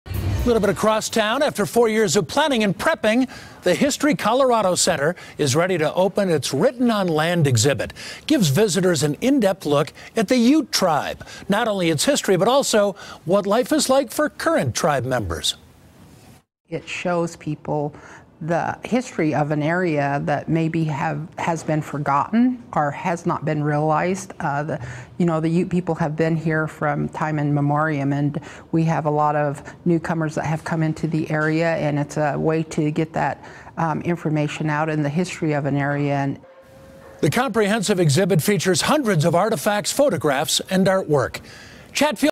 History Colorado Center Ready To Open 'Written On Land' Exhibit
The visitors get an in-depth history of the Ute Tribe.